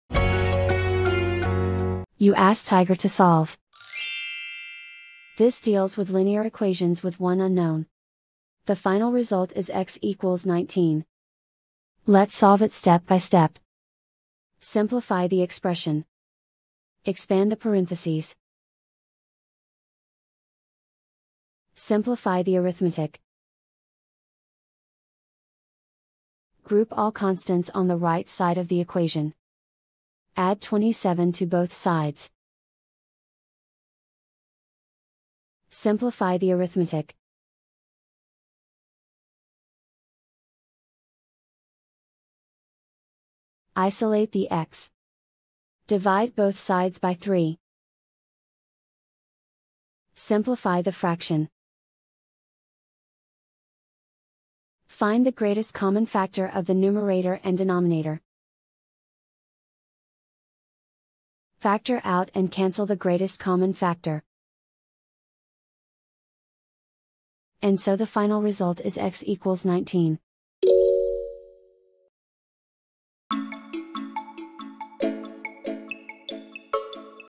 Linear equation with one unknown: Solve 3(x-9)=30 step-by-step solution
Solving 3(x-9)=30 using the linear equation with one unknown solver

for a step-by-step and more tips and tricks for dealing with linear equations with one unknown.